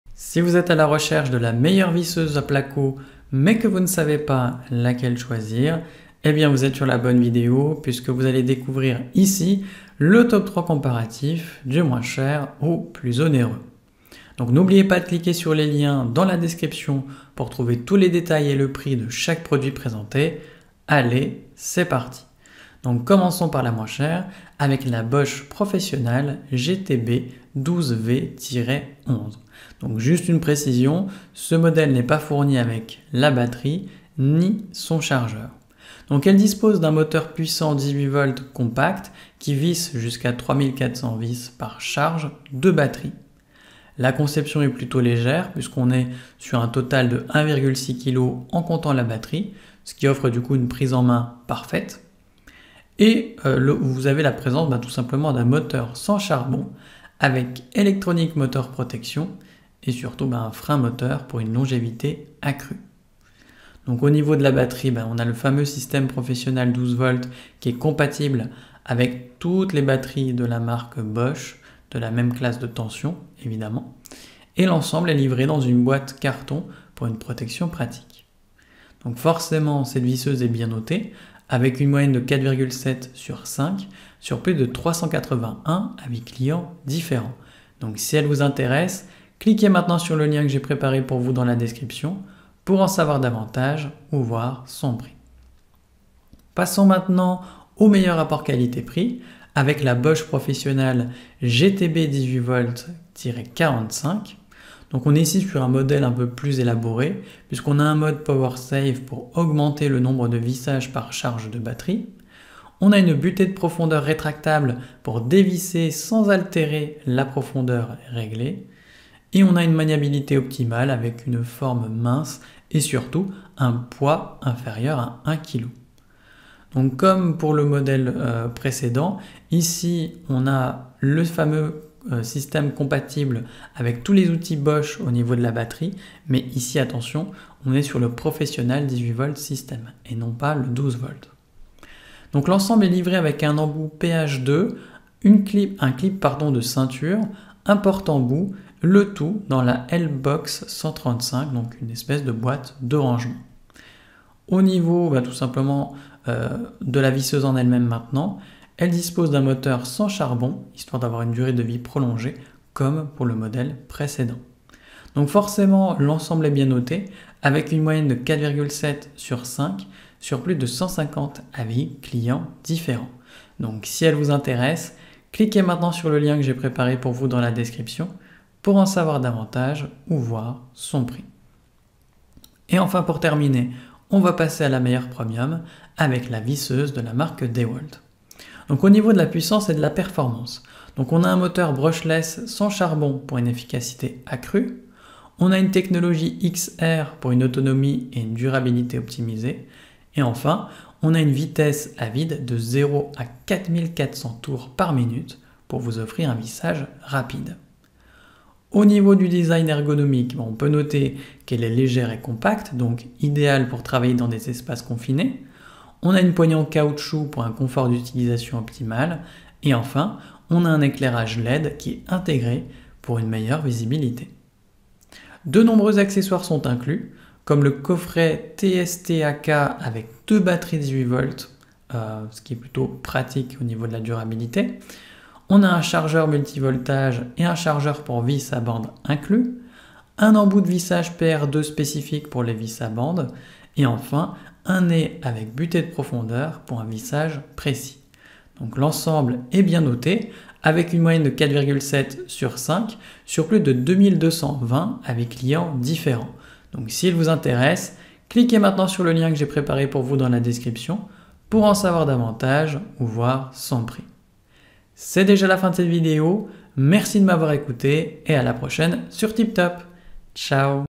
TOP 3 : Meilleure Visseuse Placo 2024 🔥
🔶 Visseuse Placo Bosch Professional GTB 12V-11 :️
🔶 Visseuse Placo Bosch Professional GTB 18V-45 :️
🔶 Visseuse Placo DEWALT :️

Vous recherchez la meilleure visseuse placo mais vous ne savez pas laquelle choisir ? Alors regardez maintenant notre comparatif du Top 3 des meilleures visseuses placo !

Pour choisir une visseuse placo, considérez la puissance, la vitesse réglable, le couple ajustable, l'ergonomie, la taille, le poids, l'autonomie de la batterie, le type de mandrin, la fiabilité de la marque, les accessoires inclus et le rapport qualité-prix. Optez pour une visseuse adaptée à vos besoins spécifiques et préférences personnelles en matière d'outillage électroportatif.

Pour bénéficier des meilleurs rapports qualité/prix, n’hésitez pas à cliquer sur les liens ci-dessus.

0:00 Quelle visseuse placo choisir ?
0:23 Visseuse Placo Bosch Professional GTB 12V-11
1:37 Visseuse Placo Bosch Professional GTB 18V-45
3:03 Visseuse Placo DEWALT
4:25 Faites maintenant votre choix !

A la prochaine sur TIP TOP ! 🤞